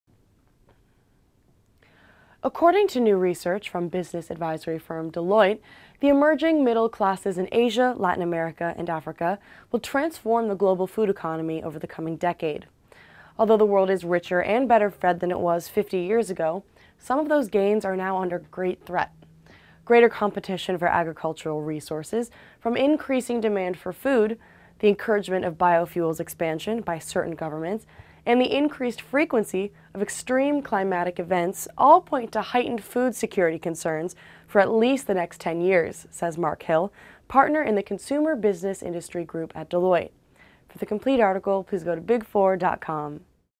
Deloitte: UK food prices and opportunities to go up
According to new research from business advisory firm Deloitte, the emerging middle classes in Asia, Latin America and Africa will transform the global food economy over the coming decade.